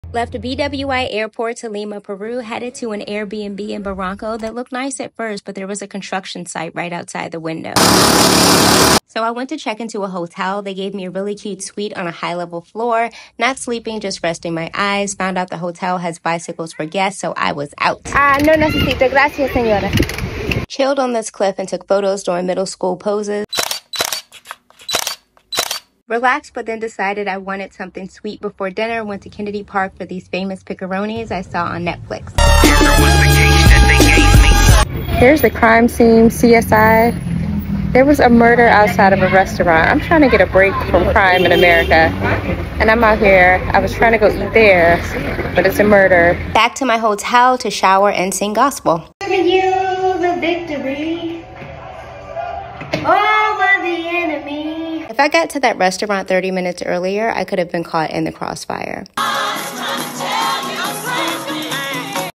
February 11, 2024
From a bad AirBNB to an amazing hotel - to coastal bicycle rides, picarones to a murder